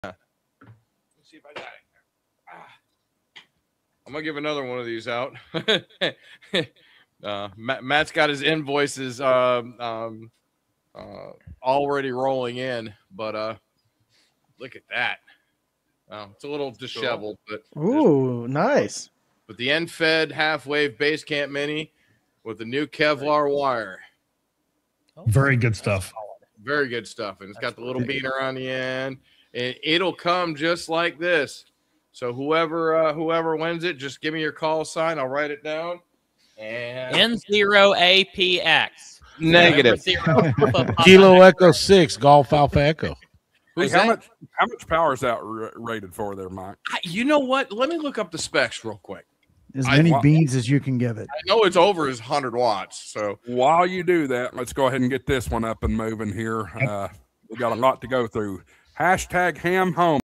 Unboxing the Beast! 🤯 Check out this rugged Kevlar EFHW antenna that just went out to a lucky op...
A close-up look at the impressive Enfed Halfway Base Camp Mini, now featuring a brand-new, nearly indestructible Kevlar wire. The power handling specs on this antenna are certainly raising eyebrows—confirming it easily handles more than 100 watts for demanding on-the-go operations. Witness the moment this piece of quality field gear heads to its new owner after a quick call sign confirmation. Pure radio excitement caught on tape!